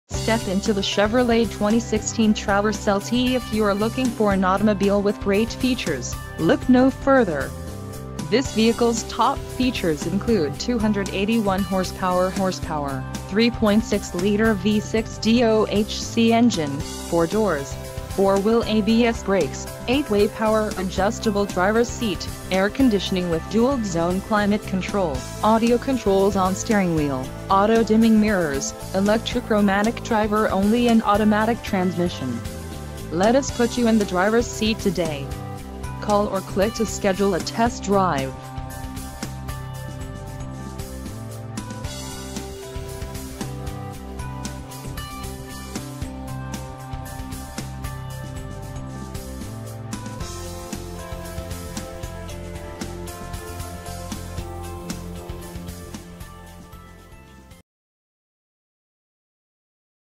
2016 Chevrolet Traverse LT
Mosaic Black Metallic 2016 Chevrolet Traverse 2LT 2LT FWD 6-Speed Automatic 3.6L V6 SIDI Exterior Parking Camera Rear.

Reviews:
 * Smooth ride even when the road isn't; can accommodate up to eight passengers; plenty of cargo space behind the third row; peace of mind that comes with top safety scores. Source: Edmunds